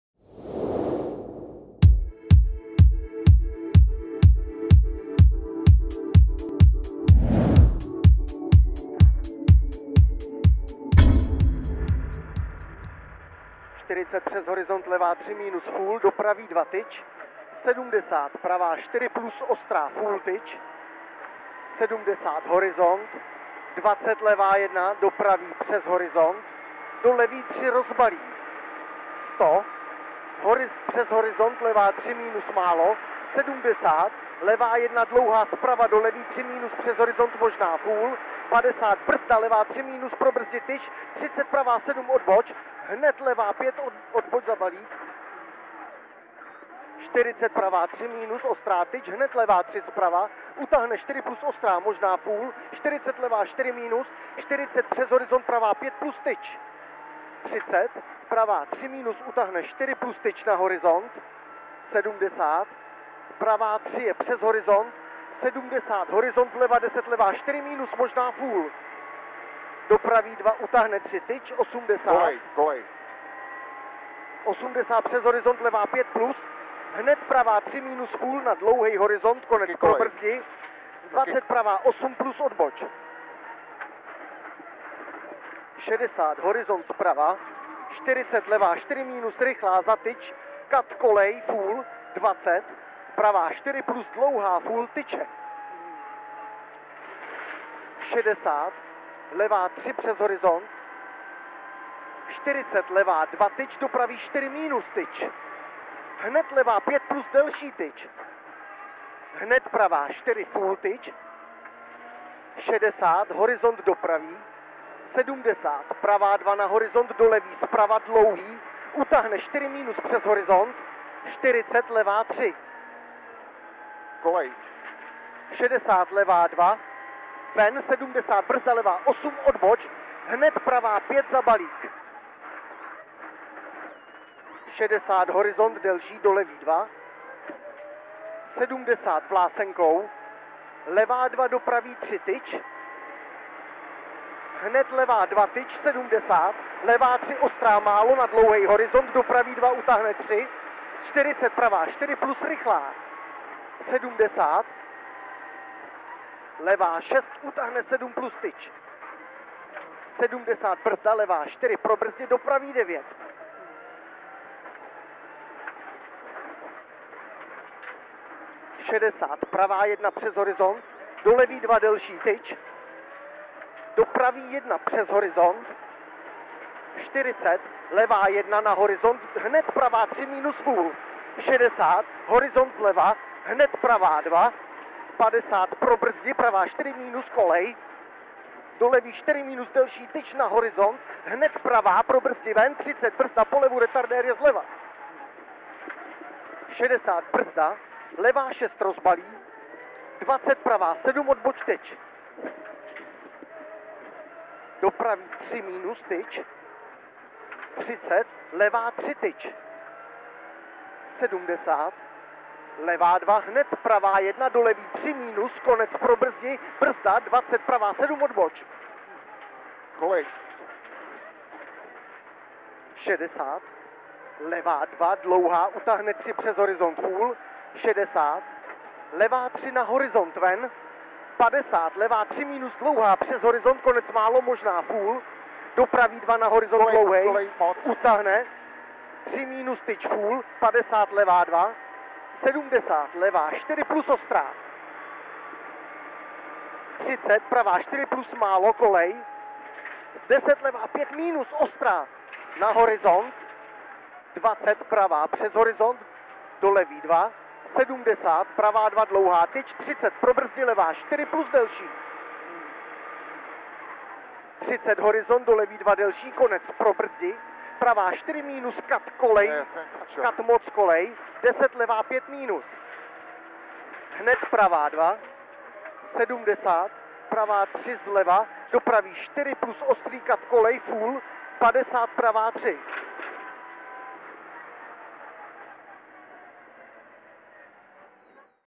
Jan Sýkora ● RZ9 ● Rallye Šumava Klatovy 2018 ● ONBOARD
Jan Sýkora a Štěpán Palivec, Mitsubishi Lancer Evo IX R4, RZ9 Strašínská